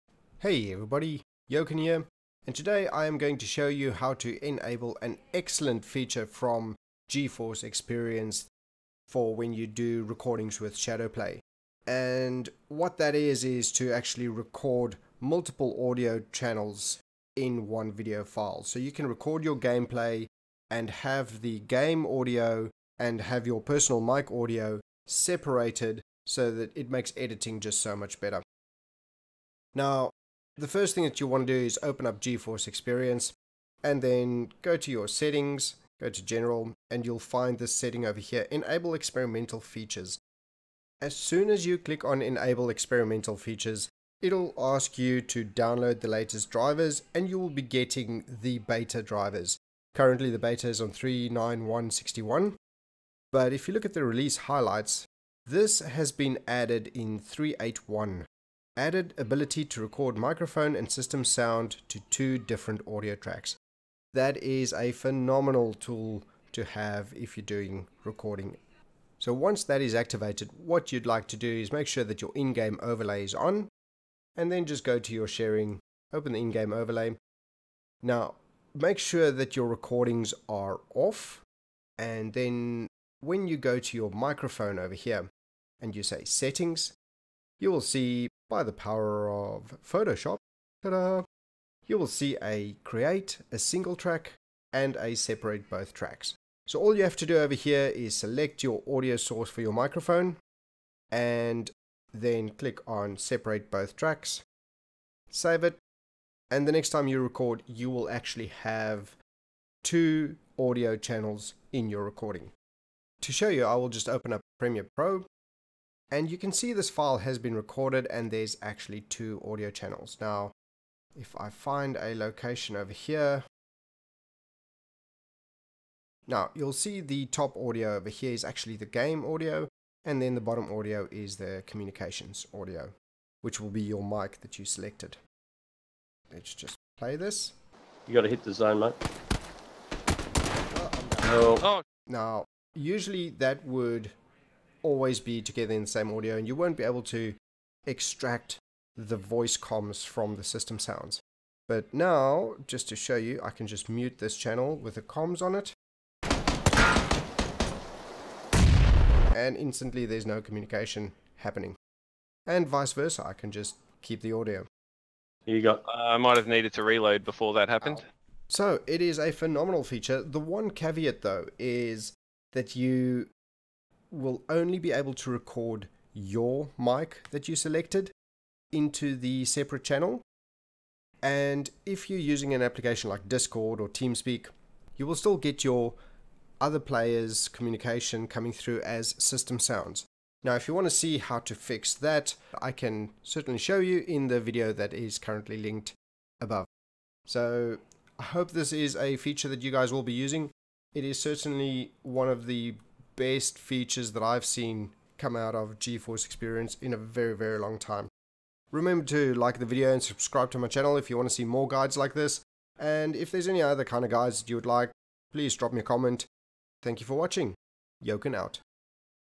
How to record audio and separate both tracks with NVidia Geforce Experience Shadowplay & Voicemeeter
Easily enable multitrack recording with the new Nvidia Geforce Experience Shadowplay 'Separate Both Tracks' feature.

This"How to record audio multitrack with NVidia Geforce Experience Shadowplay & Voicemeeter Banana" guide will show you how to enable the NVidia Geforce Experience to separate both tracks while performing Shadowplay video recording.

The latest Beta version of NVidia Geforce Experience allows you to easily activate your system audio and mic audio to separate both tracks while recording shadowplay footage.  This will work for both instant replay video recording as well as normal shadowplay video recording.

With multitrack audio recorded, you'll be able to easily edit out any unwanted sounds coming from your mic, or reduce the system audio to allow yourself to be heard over a huge explosion.

The "How to record audio multitrack with NVidia Geforce Experience Shadowplay & Voicemeeter Banana" is a quick guide on how to activate the multiple audio track recording when you record gameplay footage (or any other footage, since Shadowplay can record your desktop too).

I've been using NVidia Shadowplay  to record gameplay for a couple years now, and the Geforce Experience honestly just gets better and better. When NVidia introduced the Shadowplay feature of instant replay, I actively used it.  One thing that kept nagging me though is that Shadowplay didn't have any capability of recording multi track audio, and I ended up recording videos which had people coughing in the background etc.

Finally NVidia has graced us with the ability of recording multiple audio tracks using shadowplay!  I still use my full audio split method (seen ), but with this new feature where we can separate both tracks when recording video using shadowplay, I'm certainly capturing more "unplanned" footage now - thanks NVidia !